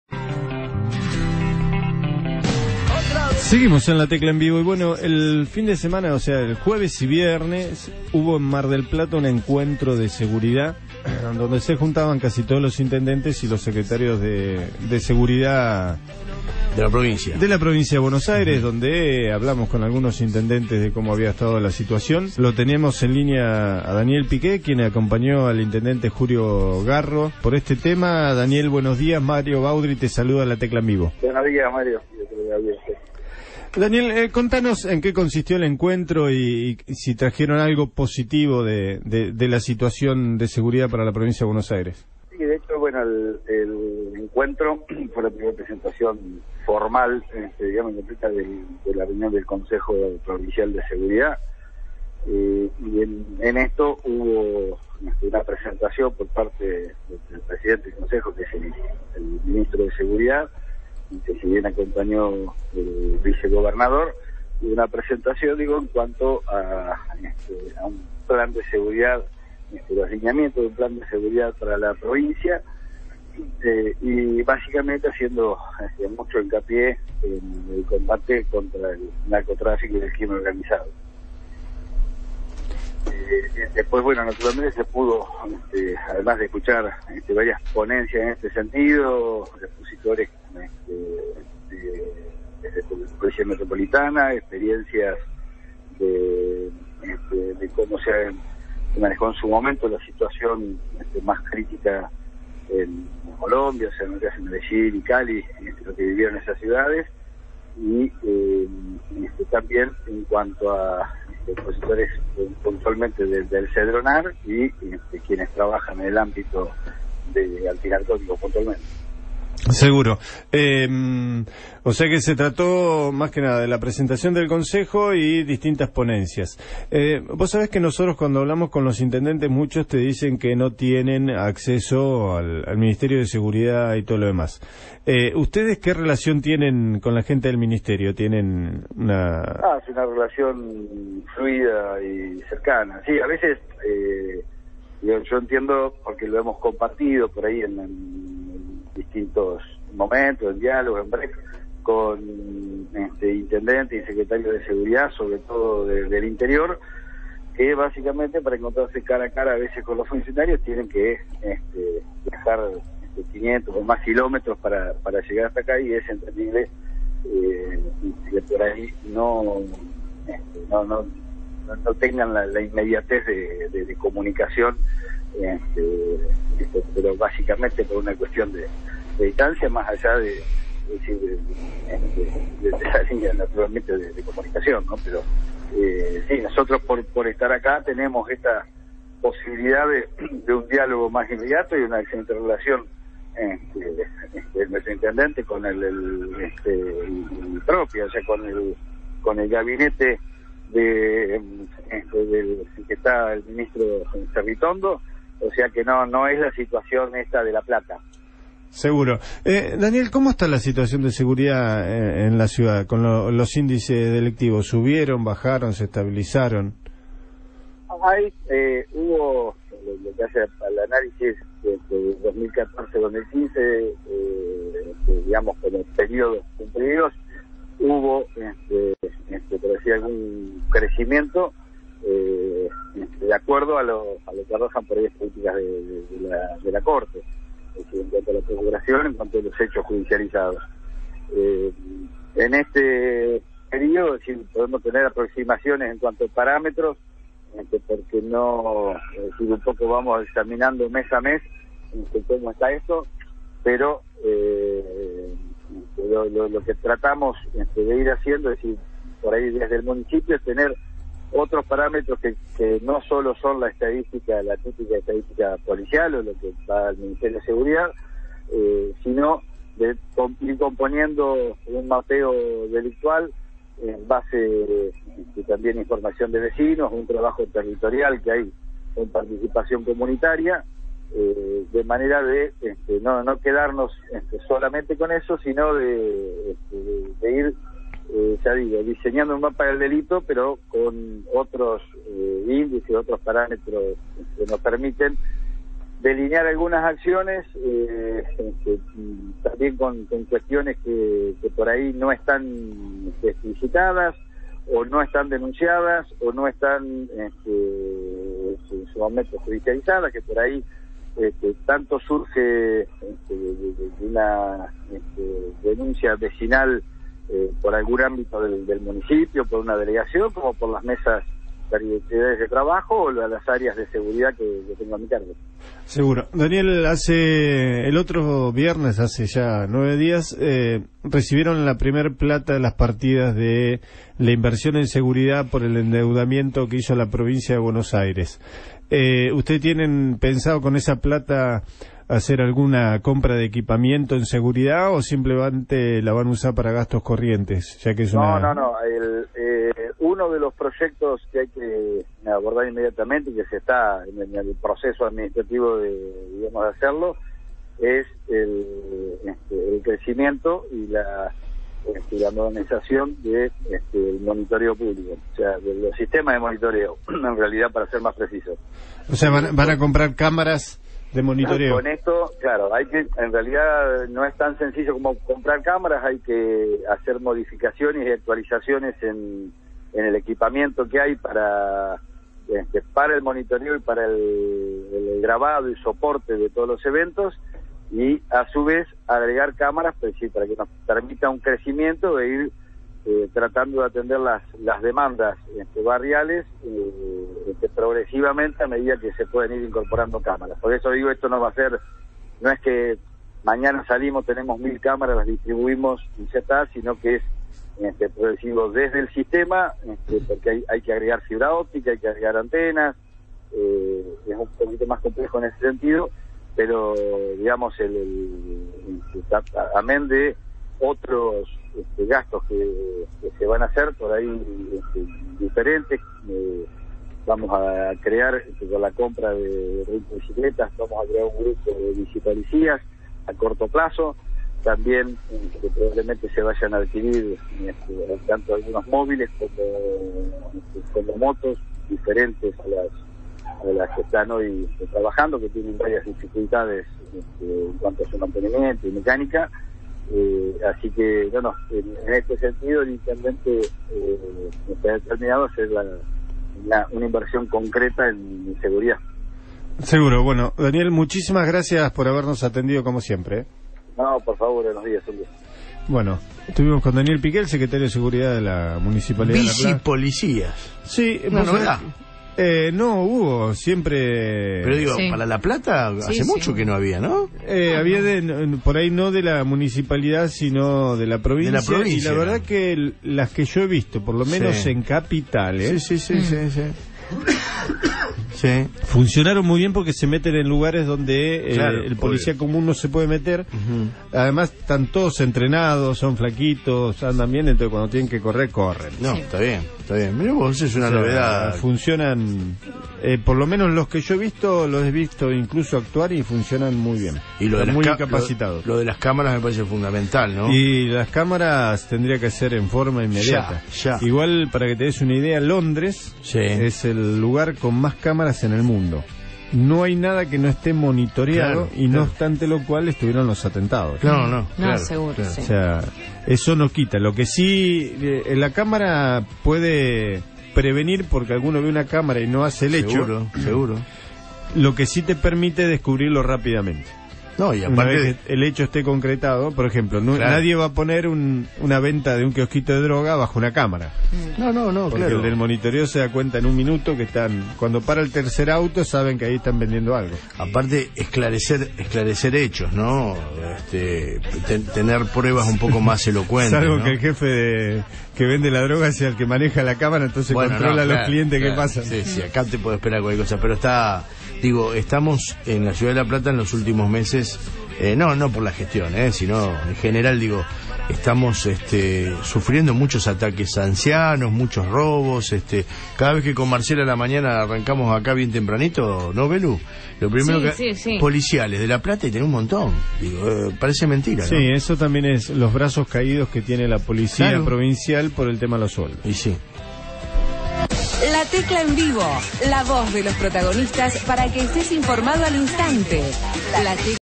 Daniel Piqué Secretario de Seguridad de la Municipalidad La Plata
La palabra de Daniel Piqué Secretario de Seguridad de la Municipalidad LaPlata por el encuentro de seguridad de la Prov. de Bs.As
" Uno de los proyectos es poner cámaras en la ciudad para tener un crecimiento en atención de denuncias barriales"